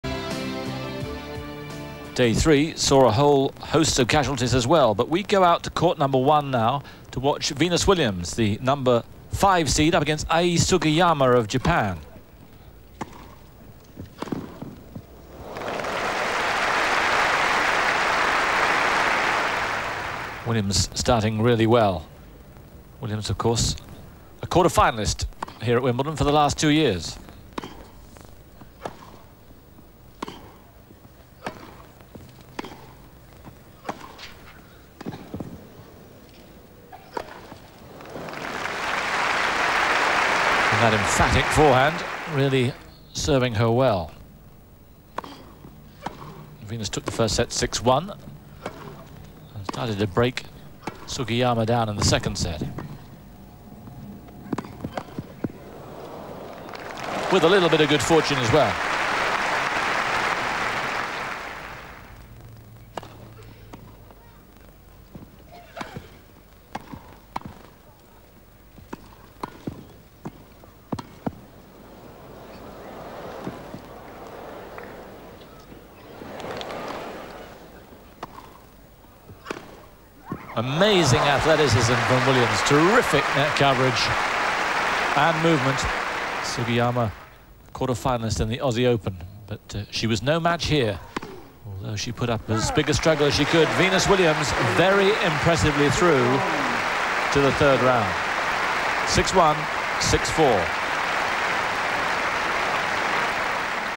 Venus Williams vs Ai Sugiyama 杉山愛 - Wimbledon 2000 2R Highlights
Wimbledon 2000
1st Round
Ai Sugiyama 杉山愛 (JPN) vs 5-Venus Williams (USA)

Please note: I do NOT take requests.